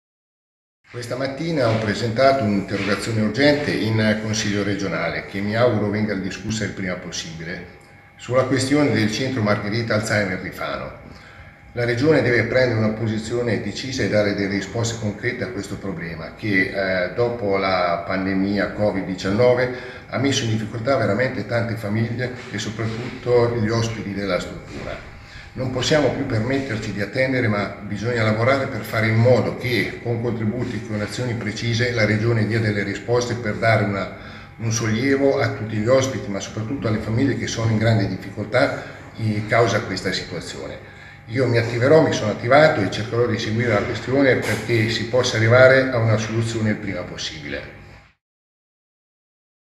Centro Margherita, Rapa: "Regione dia risposte urgenti"
“La Regione dia risposte urgenti sulla riapertura del centro Margherita”. E’ quanto chiede il consigliere regionale Boris Rapa che oggi ha presentato un’interrogazione.